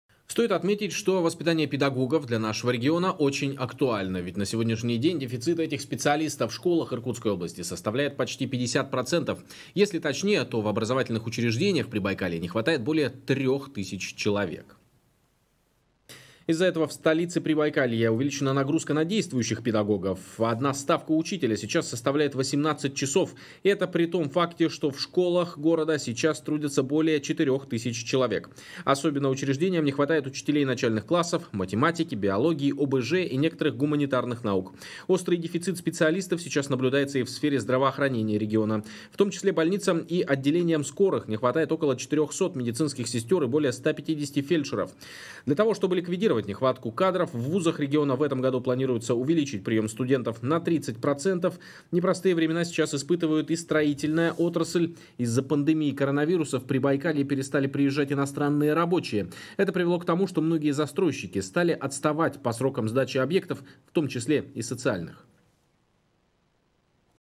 Наибольший дефицит кадров в регионе зафиксирован в сфере образования,здравоохранения и строительства
Воспитание педагогов для нашего региона очень актуально. Ведь на сегодняшний день дефицит этих специалистов в школах Иркутской области составляет почти 50 процентов. Если точнее, то в образовательных учреждениях Прибайкалья не хватает более трёх тысяч человек.

Из-за этого в столице Прибайкалья увеличена нагрузка на действующих педагогов. Одна ставка учителя сейчас составляет 18 часов! И это при том факте, что в школах города сейчас трудятся более четырёх тысяч человек. Особенно учреждениям не хватает учителей начальных классов, математики, биологии, ОБЖ и некоторых гуманитарных наук. Острый дефицит специалистов сейчас наблюдается и в сфере здравоохранения региона. В том числе больницам и отделениям скорых не хватает около 400 медицинских сестёр и более 150 фельдшеров. Для того, чтобы ликвидировать нехватку кадров, в ВУЗах региона в этом году планируется увеличить приём студентов на тридцать процентов. Непростые времена сейчас испытывает и строительная отрасль. Из-за пандемии коронавируса в Прибайкалье перестали приезжать иностранные рабочие. Это привело к тому, что многие застройщики стали отставать по срокам сдачи объектов, в том числе и социальных.